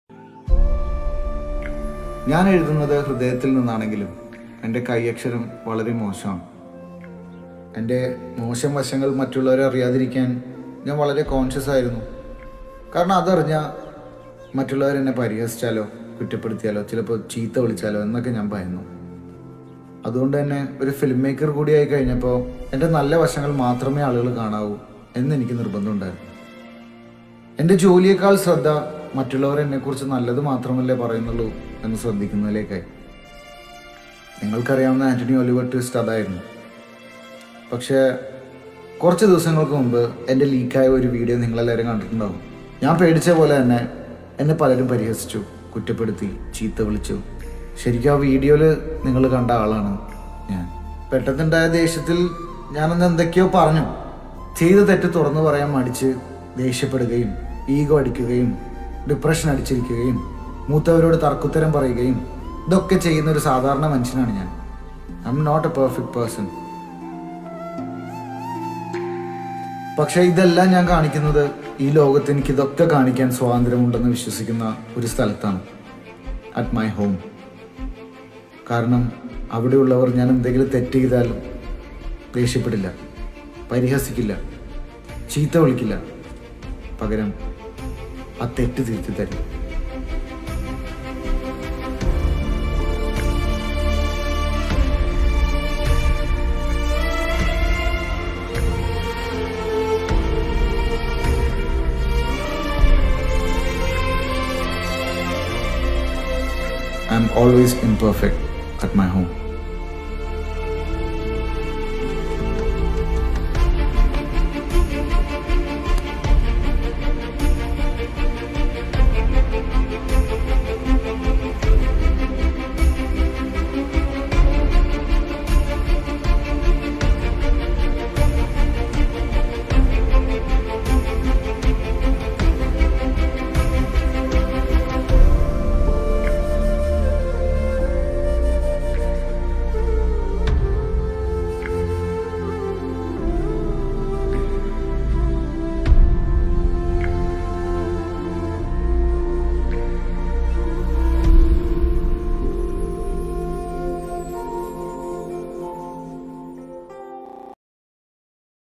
Worlds First graph art | home |2021 Indrans |Manju Pillai |Sreenath Bhasi | Naslen K Gafoor
This is the first poster art done on a single graph using 4 colours , It is completed with 6days and it took about 11hour and 4 minutes to complete the entire potrait